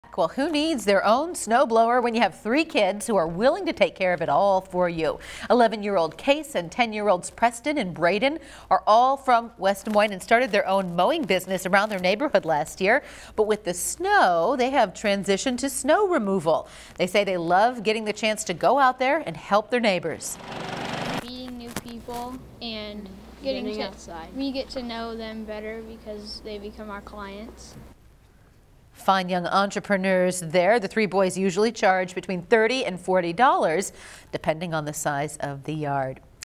West Des Moines kids create snow removal business
Meet 11-year-old Case and 10-year-olds Preston and Brayden, who have gone from mowing lawns to shoveling snow.